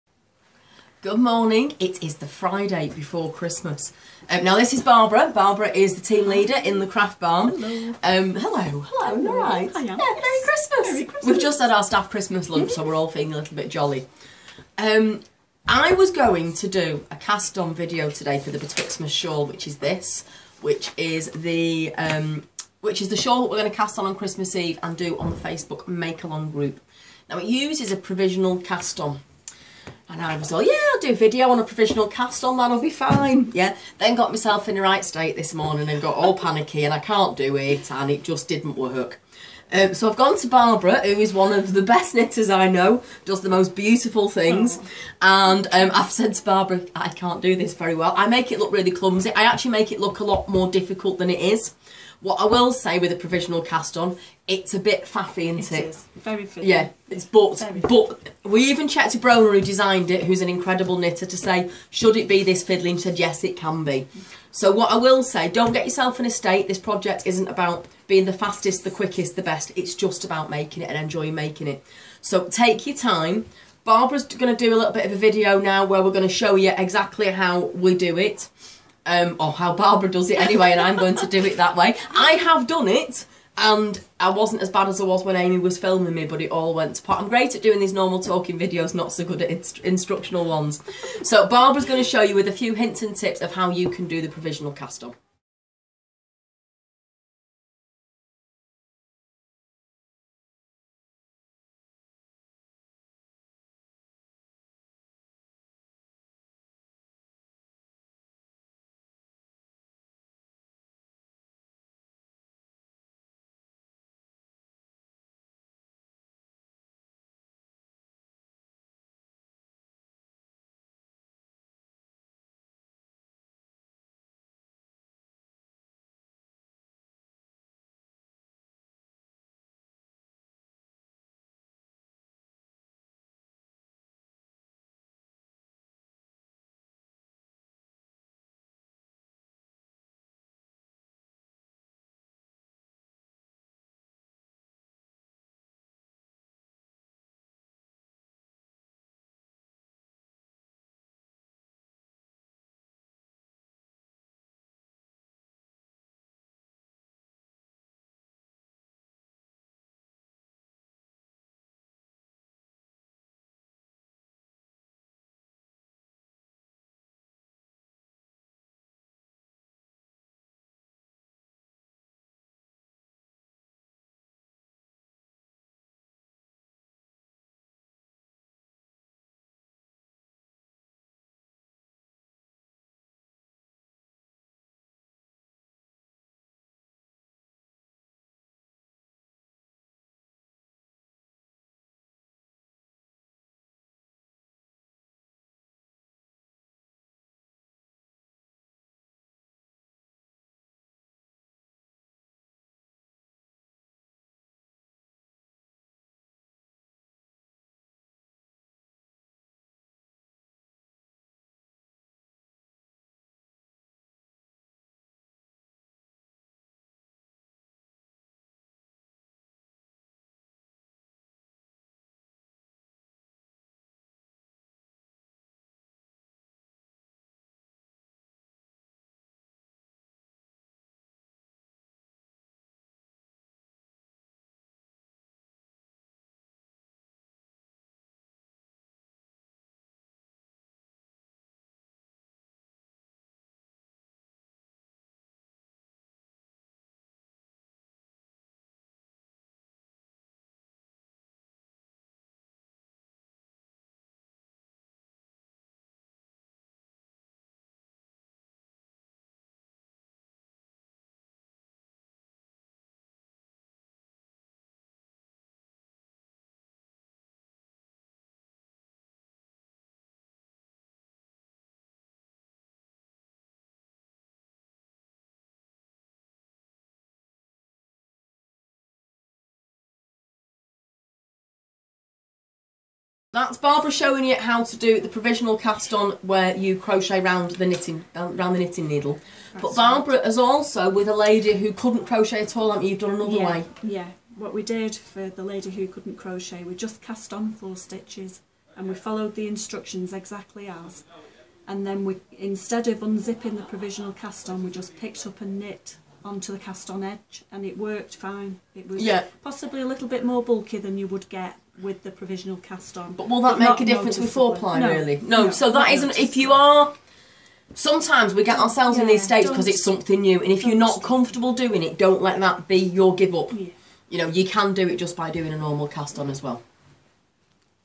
Betwixtmas Shawl Casting On
Sara and Barbara are showing you the Betwixtmas shawl cast on with a crochet hook. There is a slight mishap towards the end where the yarn ends up in the middle, but this is fixed quite easily. We thought we would leave this bit in so you can see that knitting isn't always perfect first time!